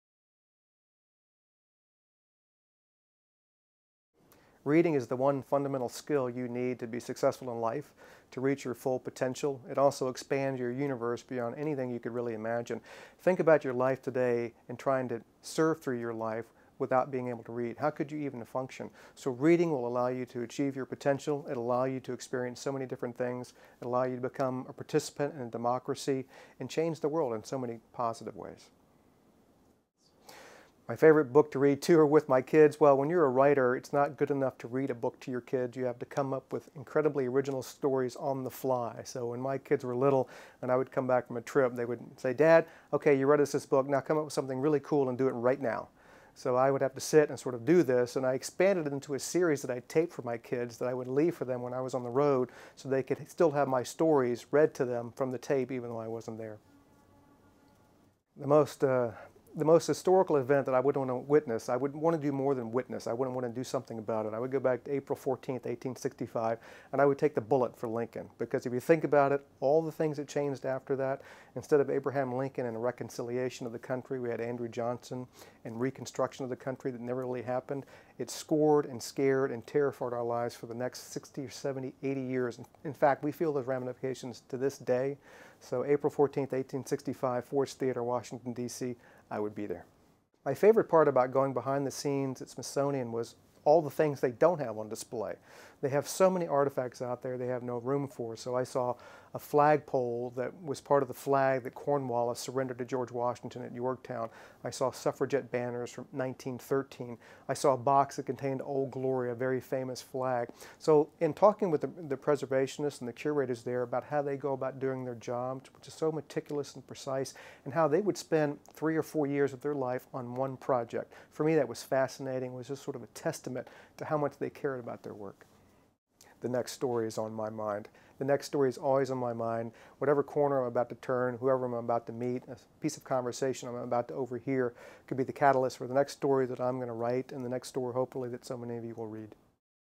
5 Questions with David Baldacci
We sat down with David Baldacci, author of The 39 Clues: Day of Doom and host of Scholastic's recent virtual field trip, "Decoding History". David shares his thoughts on reading and what it was like to host the virtual field trip at the Smithsonian's National Museum of American History.